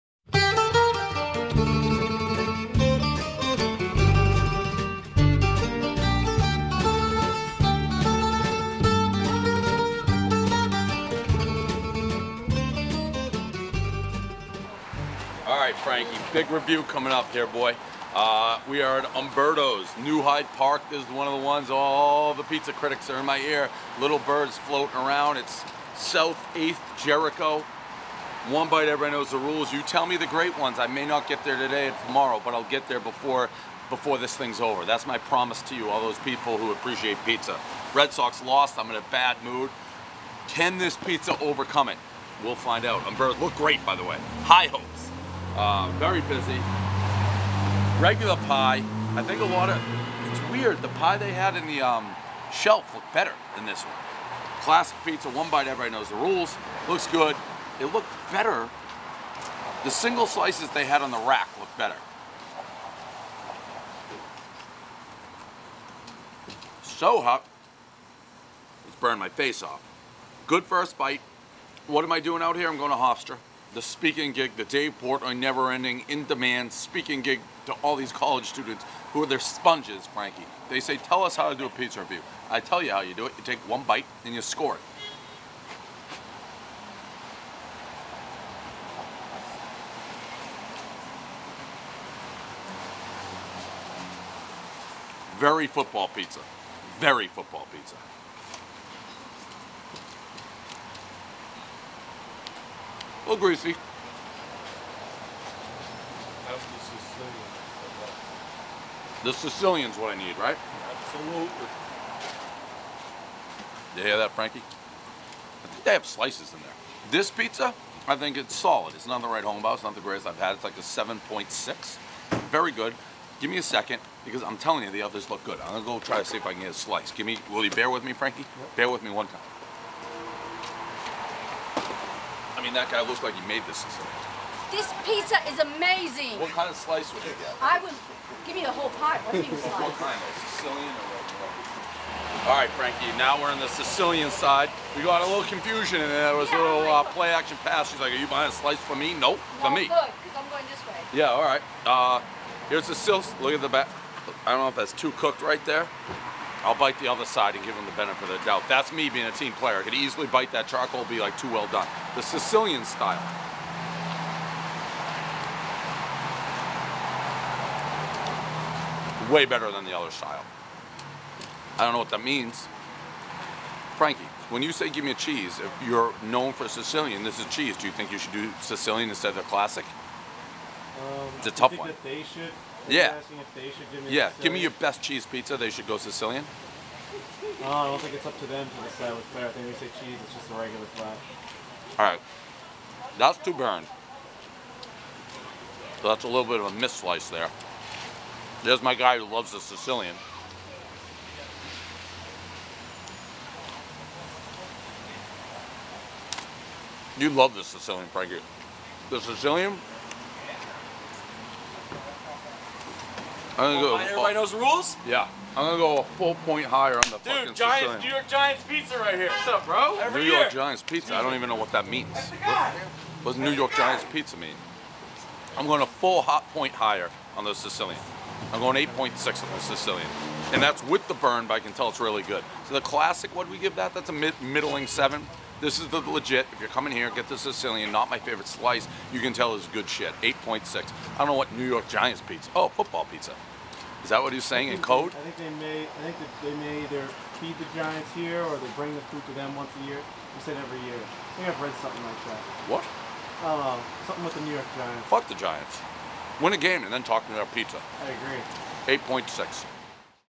Barstool Pizza Review - Umberto's (New Hyde Park,NY)
El Pres goes to famous Long Island pizza joint in New Hyde Park, Umberto's.